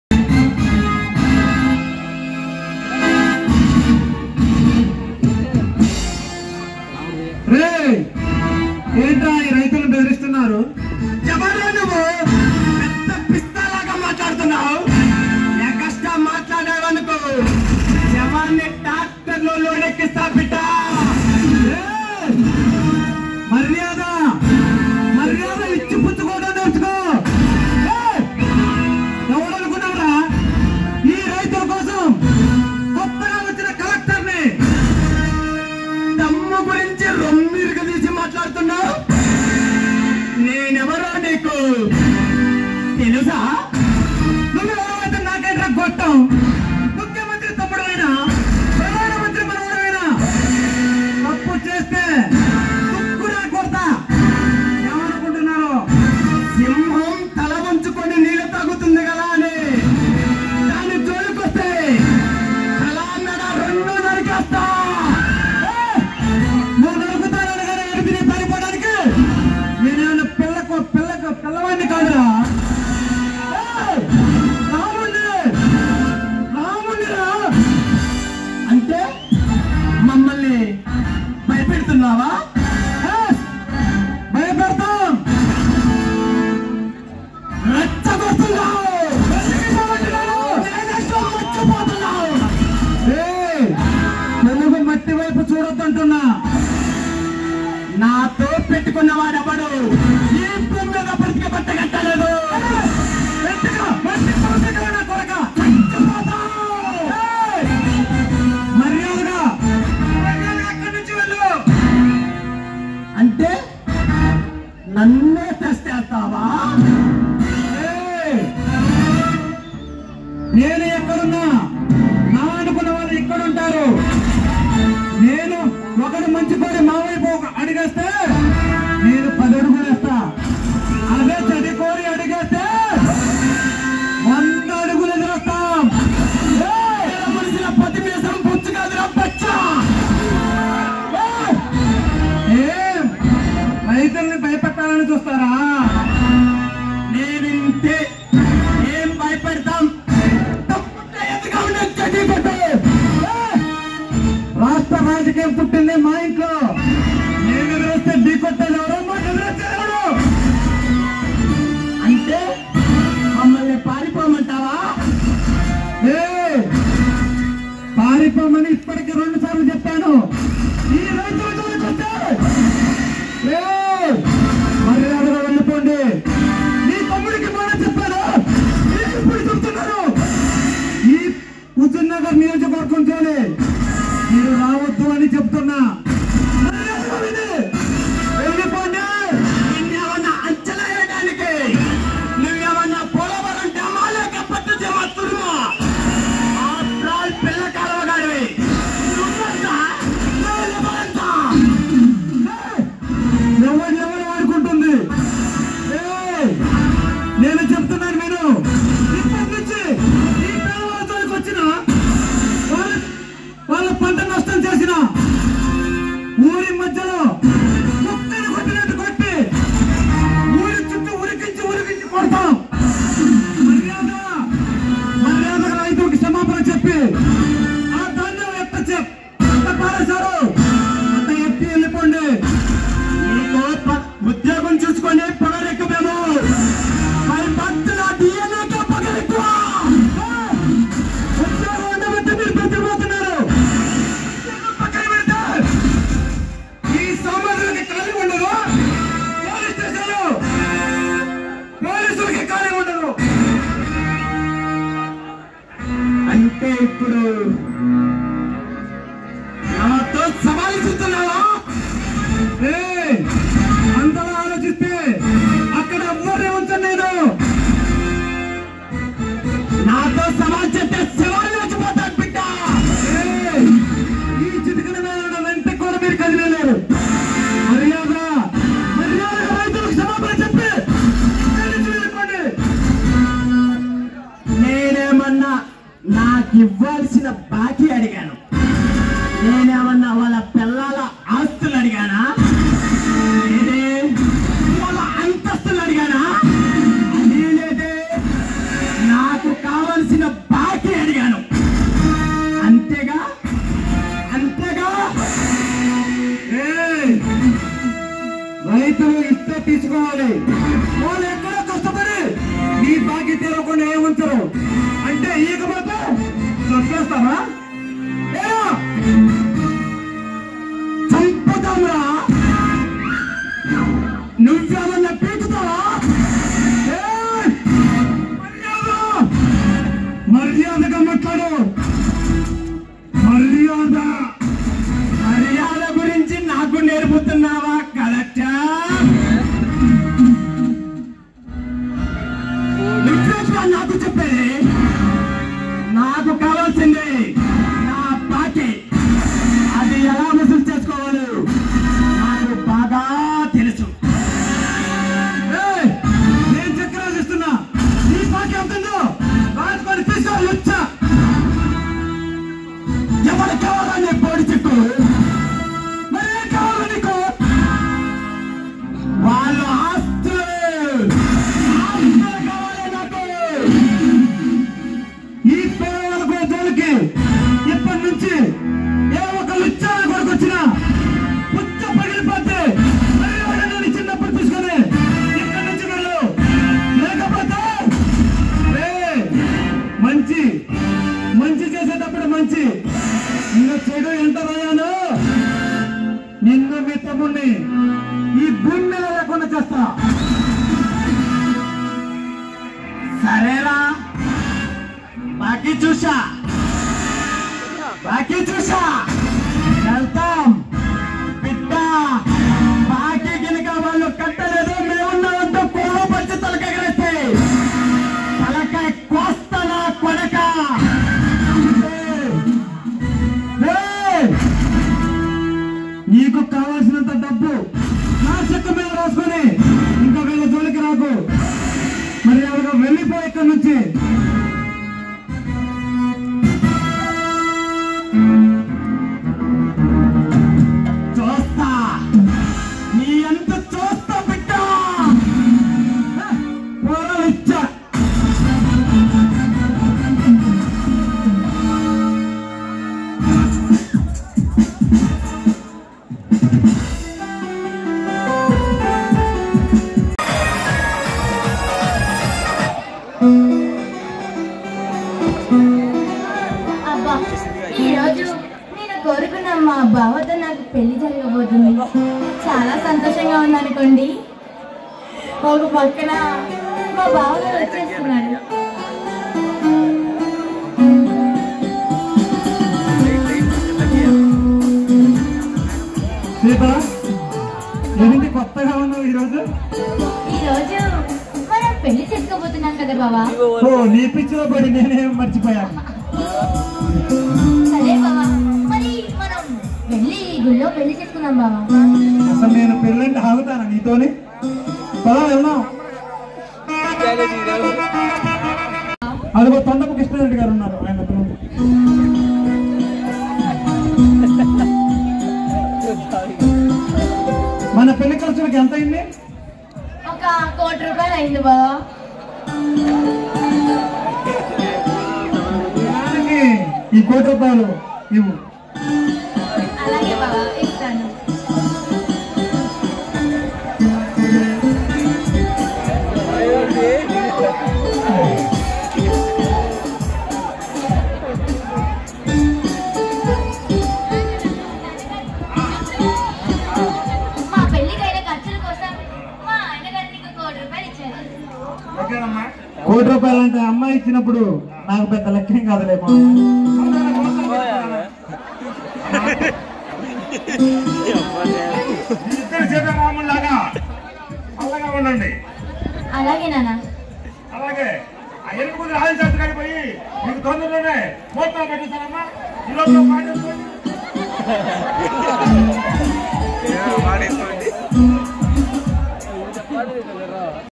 Telugu latest drama stage performance at chintriyala village suryapeta district 2023|vvr telugu vlog
Telugu natakam stage performance show in chintriyala village chinthalapalem mandalam suryapeta district